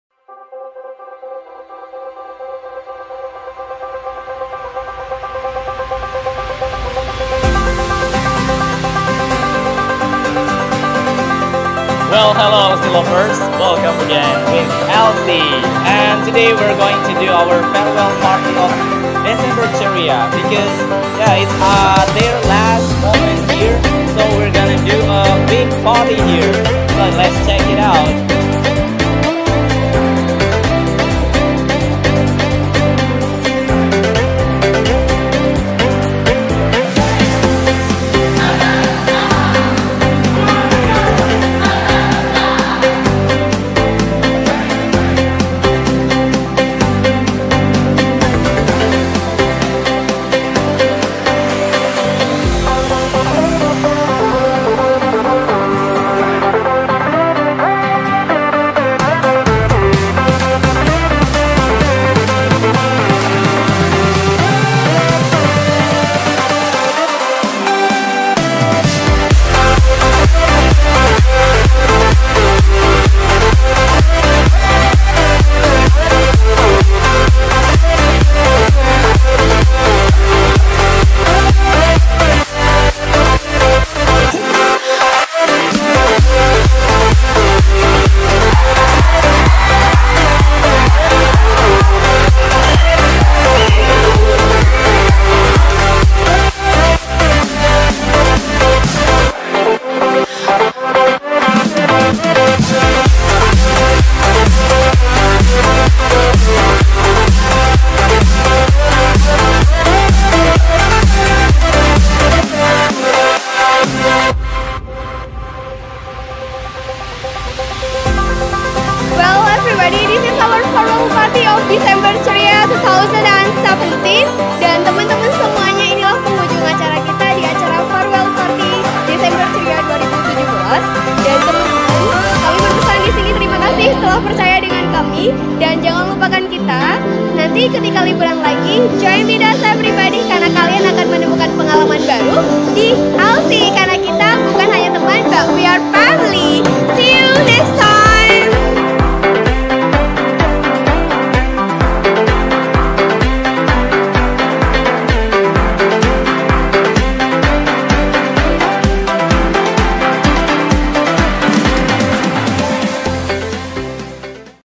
Farewell Party Program Holiday Desember Ceria 2017 Kampung Inggris LC
Liburan di LC Kampung  Inggris itu seru banget. Bukan hanya kelas belajar nya aja yang seru, sampai acara farewell party nya pun berjalan dengan sangat seru dan FUN. Seperti acara farewell party program holiday Desember 2017 ini.